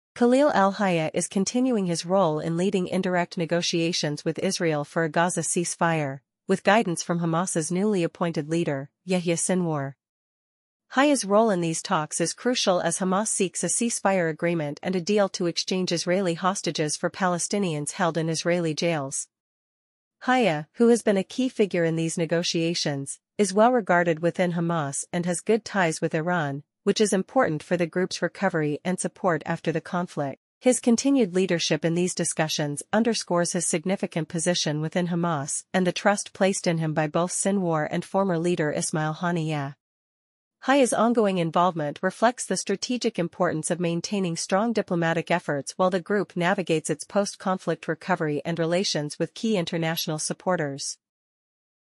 Hayya Maintains Strategic Role in Hamas as Sinwar Takes Command
As Yahya Sinwar takes over as the leader of Hamas, Khalil al-Hayya is expected to continue his significant role within the organization. Sinwar, known for his hardline stance, has already influenced military decisions in Gaza and is now set to lead both the political and military wings of Hamas. Hayya, trusted by Sinwar, will likely remain crucial in negotiating ceasefires and managing external relations, particularly with Israel and other regional actors.